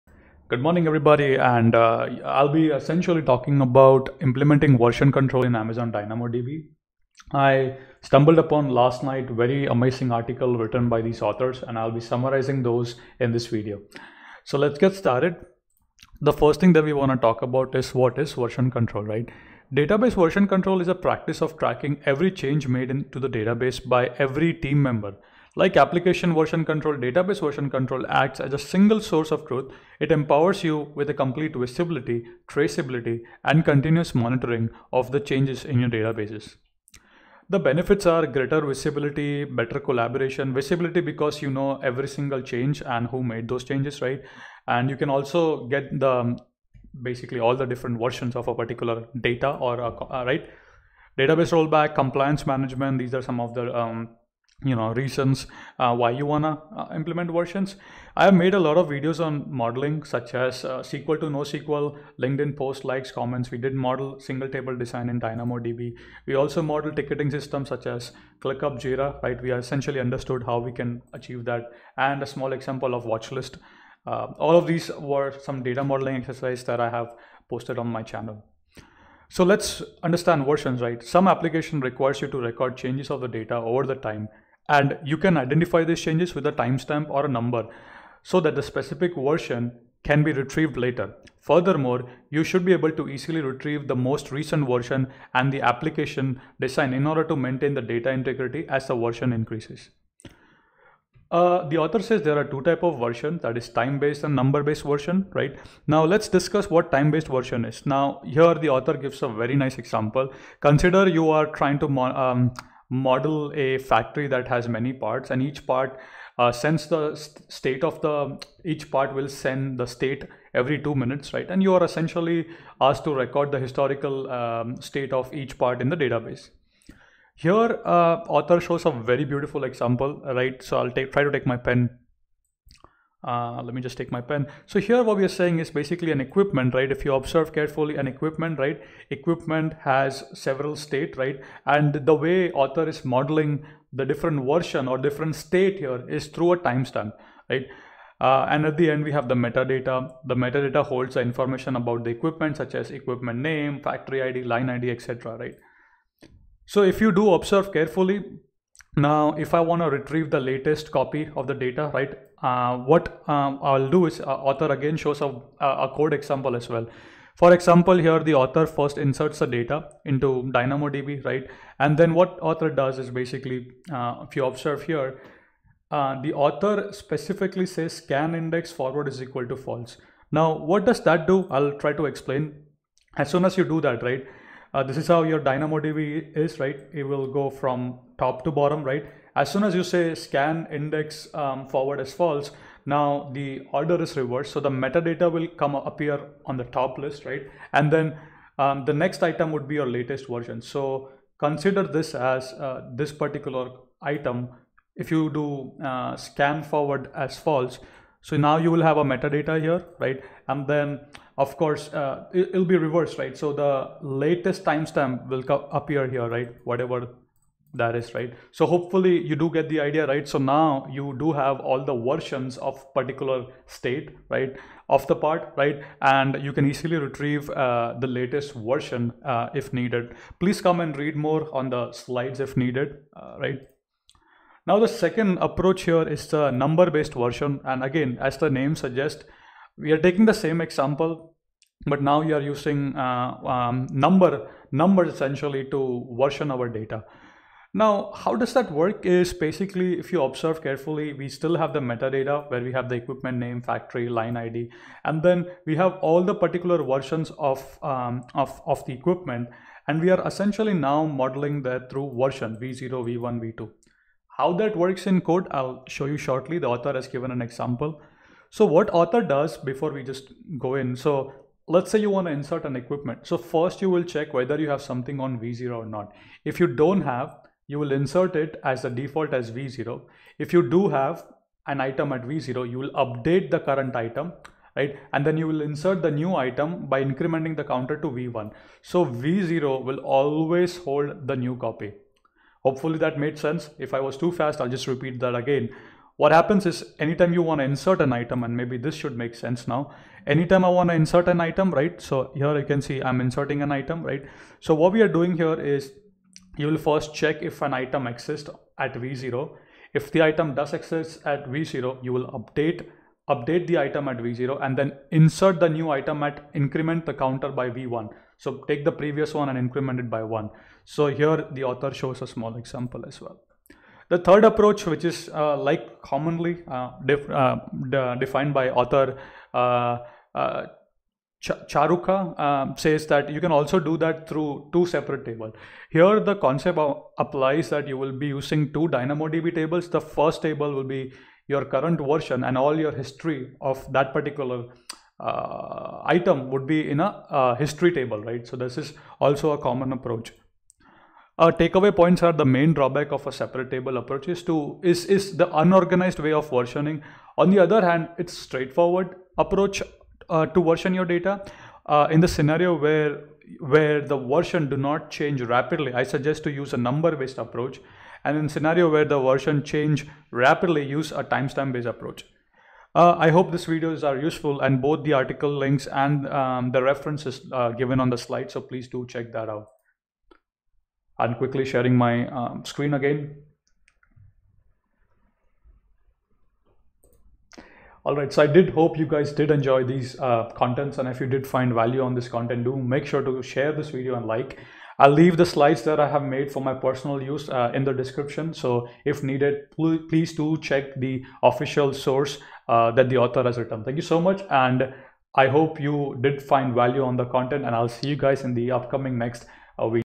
Implementing version control using Amazon DynamoDB | TimeStamp based VS Number Based Approach
aws. “Implementing Version Control Using Amazon DynamoDB.” Accessed October 1, 2022.

lightrun. “Top 8 Database Version Control Tools.” Accessed October 1, 2022.

dynobase. “How-To Implement DynamoDB Data Versioning (The Right Way).” Accessed October 2, 2022.

Thank you, Samaneh Utter and  Charuka Herath  for amazing articles